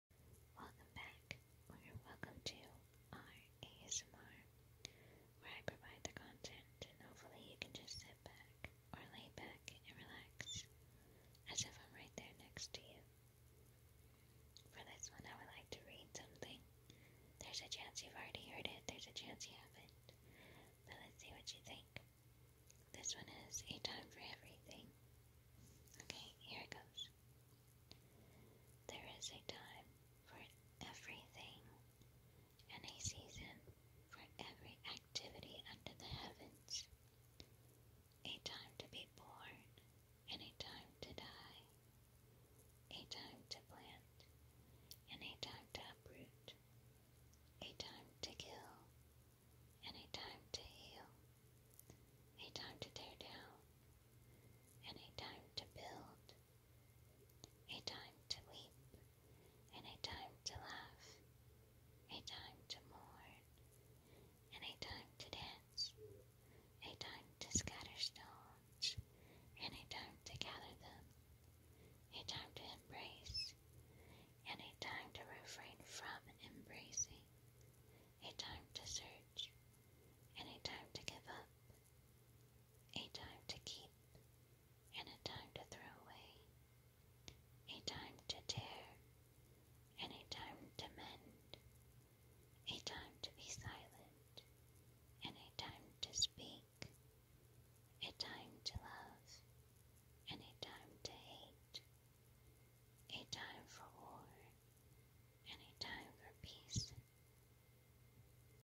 [ASMR] whispered reading with blackscreen to help relax/fall asleep
Thank you so much for being a part of my day and for letting me be a part of yours!